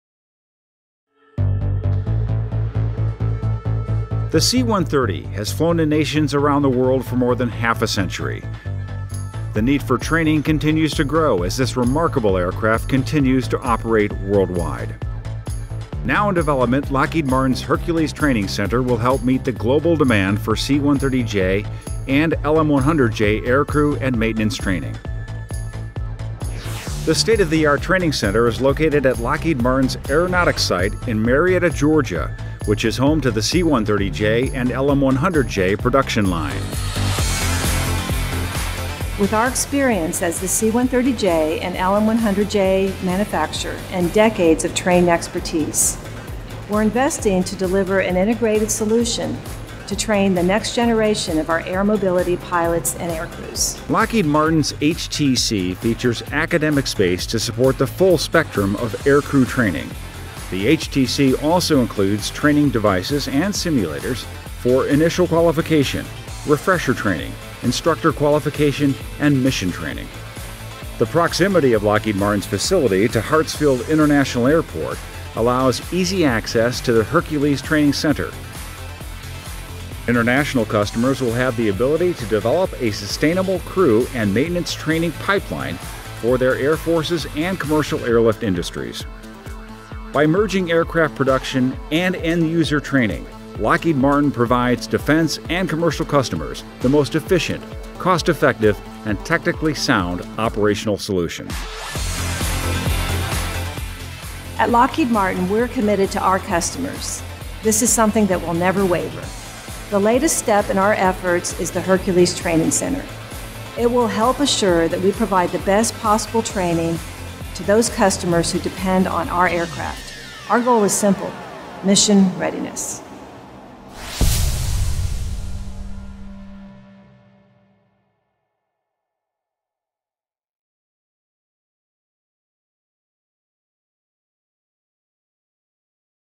Lockheed Martin to Open Hercules Training Center
Now under construction, Lockheed Martin’s Hercules Training Center (HTC) will train the next generation of air mobility pilots and crews for the C-130J and LM-100J. Located adjacent to aircraft production in Marietta, Georgia, the HTC will include classroom space, training devices and a new, reconfigurable C-130J / LM-100J full mission simulator. For additional information, visit our websites